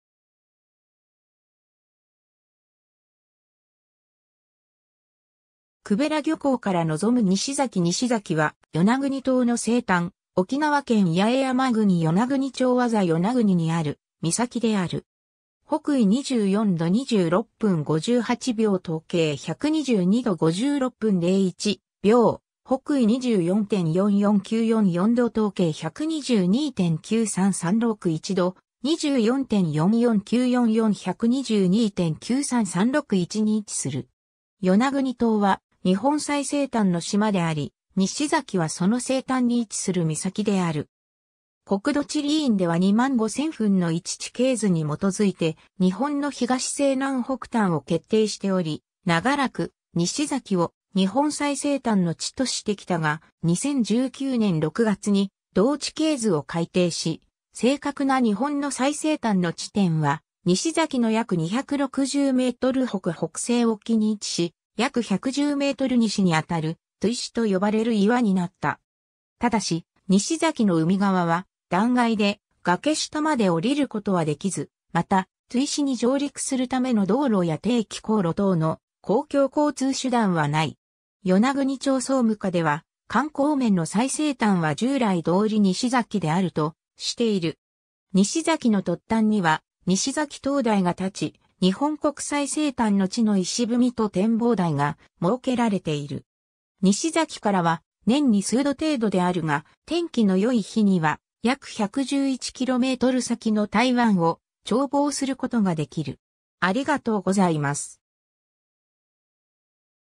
西崎
久部良漁港から望む西崎 西崎（いりざき、いりさてぃ）は、与那国島の西端、沖縄県八重山郡与那国町字与那国にある岬である。
 北緯24度26分58秒 東経122度56分01秒 / 北緯24.44944度 東経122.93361度 / 24.44944; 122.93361に位置する。
 与那国島は日本最西端の島であり、西崎はその西端に位置する岬である。
 国土地理院では2万5千分の1地形図に基づいて日本の東西南北端を決定しており、長らく西崎を日本最西端の地としてきたが、2019年6月に同地形図を改訂し、正確な日本の最西端の地点は、西崎の約260m北北西沖に位置し、約110ｍ西にあたる「トゥイシ」と呼ばれる岩になった。
 ただし、西崎の海側は断崖で崖下まで降りることはできず、また、トゥイシに上陸するための道路や定期航路等の公共交通手段はない。
与那国町総務課では、観光面の最西端は従来通り西崎であるとしている。
 西崎の突端には西埼灯台が立ち、「日本国最西端之地」の碑と展望台が設けられている。
西崎からは、年に数度程度であるが、天気のよい日には約111km先の台湾を眺望することができる。